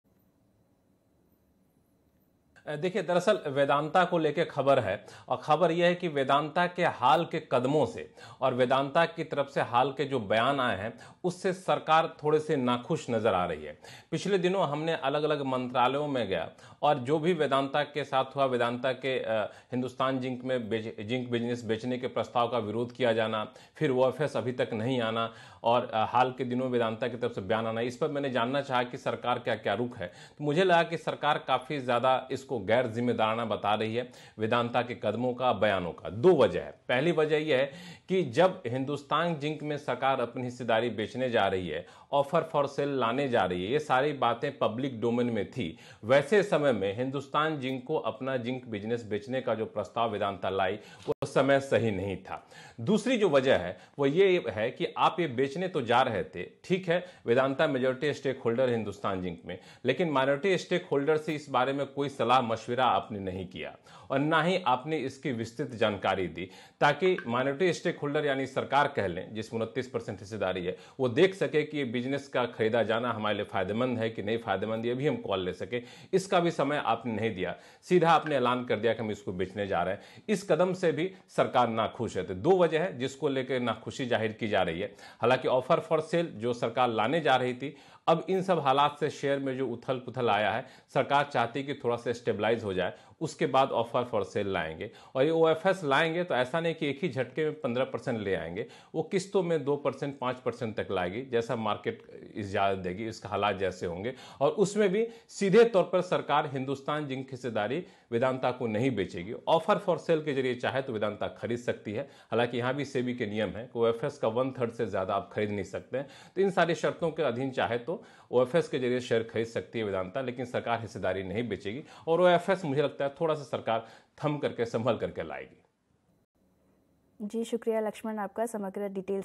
Vedantના નિર્ણયો અને નિવેદનથી સરકાર નાખુશ, નાના રોકાણકારોના હિતમાં સરકારે વિરોધ નોંધાવ્યો
CNBCBajar |BreakingNews| વેદાંતાના નિર્ણયો અને નિવેદનથી સરકાર નાખુશ, નાના રોકાણકારોના હિતમાં સરકારે વિરોધ નોંધાવ્યો